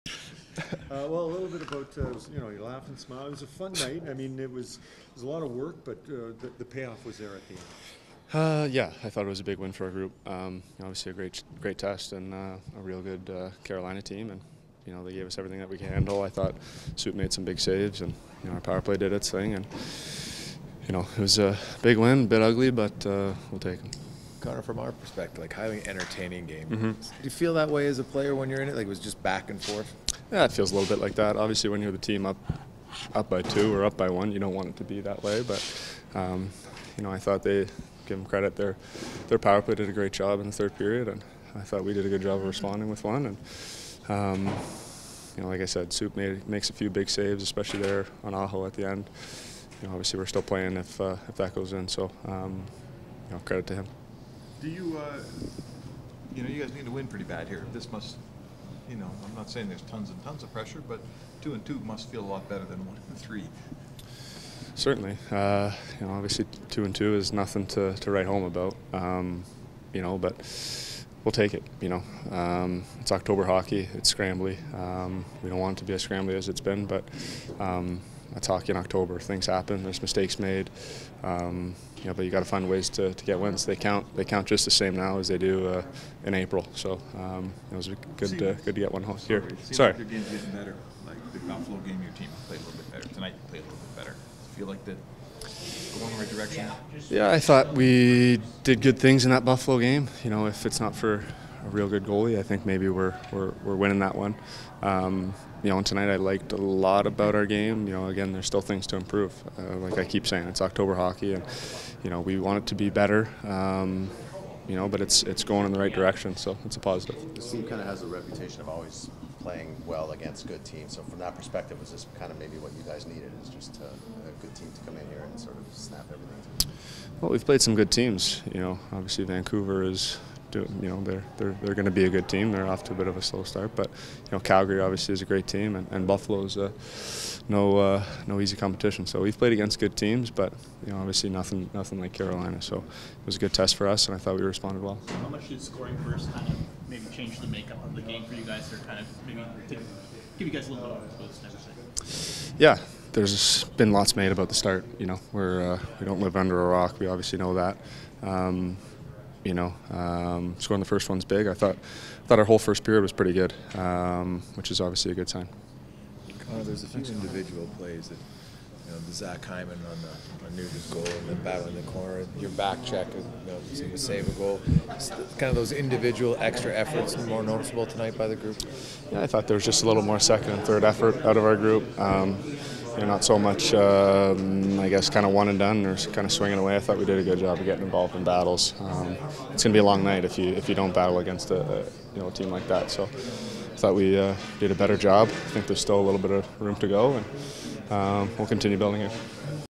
POST-RAW | Connor McDavid 10.20.22
Connor talks to the media after the Oilers defeated the Hurricanes 6-4 on Thursday night.